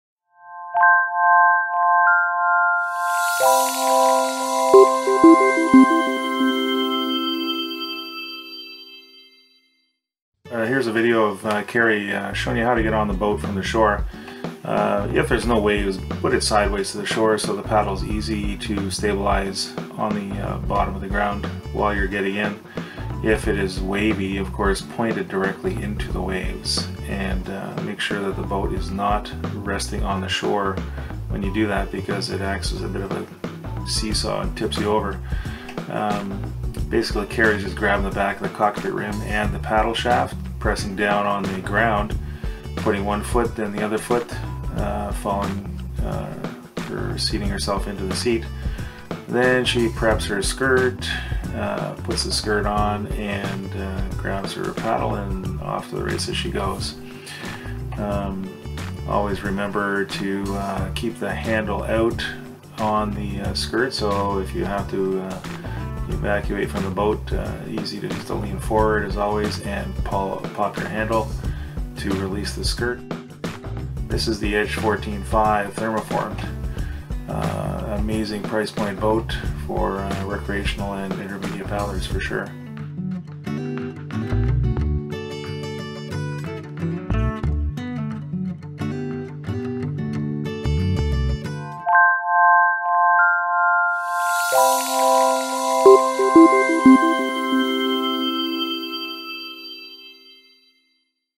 Getting in (and out) of your kayak from shore
We show you here how to use your paddle to stabilize you and your boat while getting in from shore.  To get out of your kayak again, just reverse the entire process seen here.  Have fun and see you out on the water!

Have fun and see you on the water!